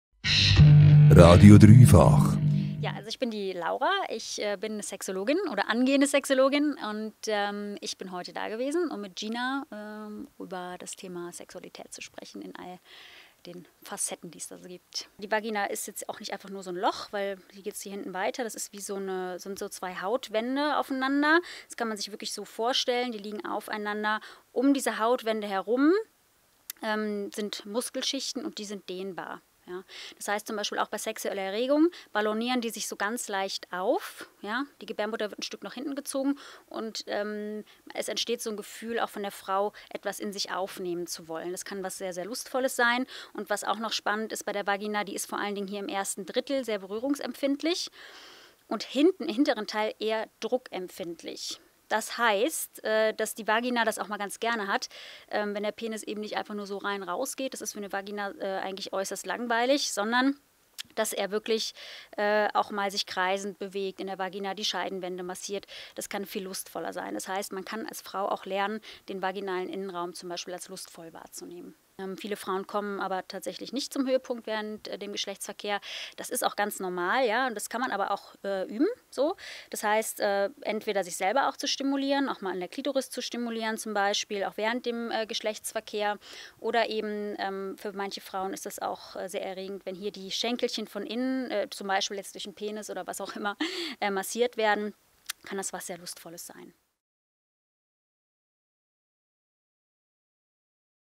Gspröchsstoff: Sex ist nicht nur Sex
Laura Burkhardt ist studierte Sexologin und überzeugt, dass Sexualität weit mehr als nur unser Schlafzimmer betrifft. Was es mit dem 'Loch' Vagina auf sich hat, erklärt sie im Video.